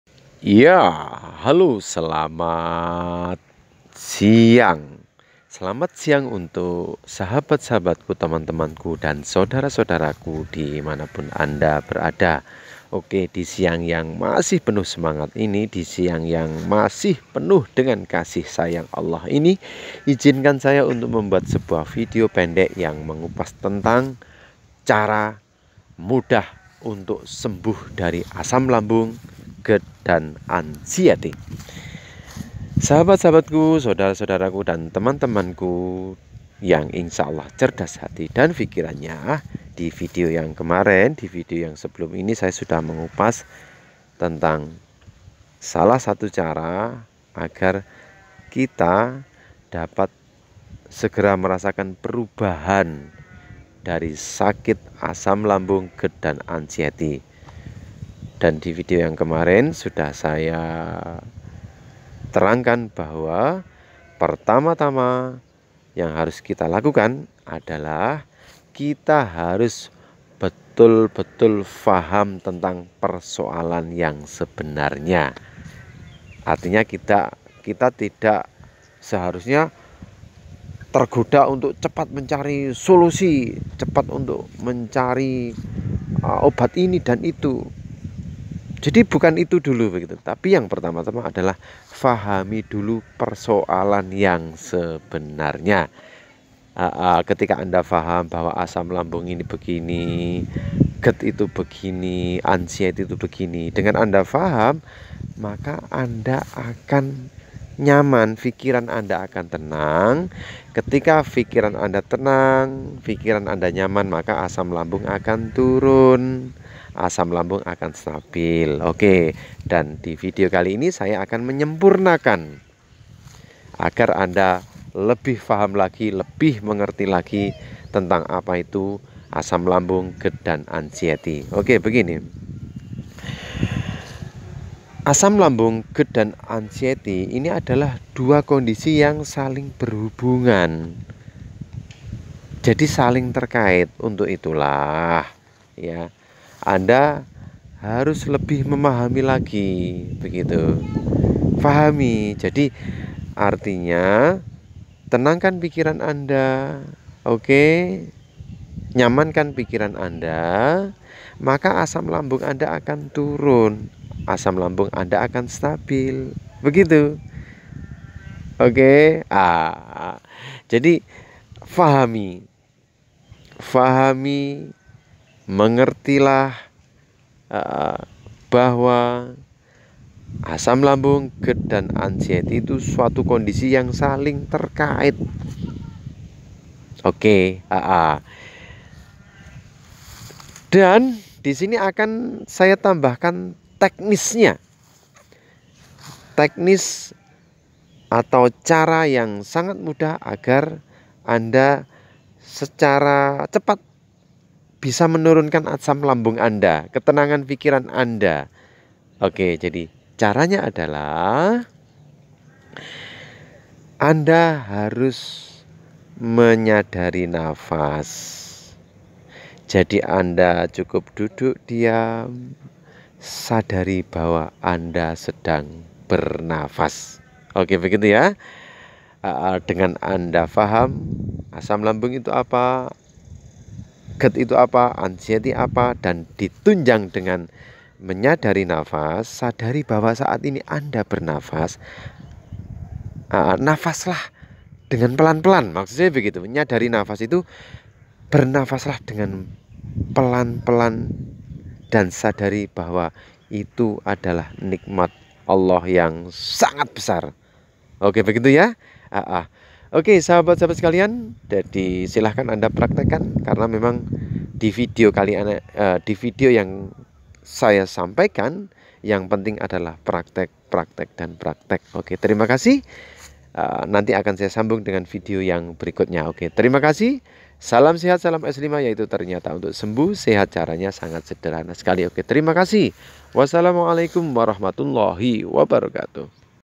CARA MUDAH SEMBUH DARI ASAM LAMBUNG GERD DAN ANXIETY
dengan faham persoalan dan menyadari bahwa anda sedang bernafas/menyadari nafas maka anda akan menjadi rileks santai dan tenang akhirnya asam lambung gerd dan anxiety anda akan membaik konsultasi Wa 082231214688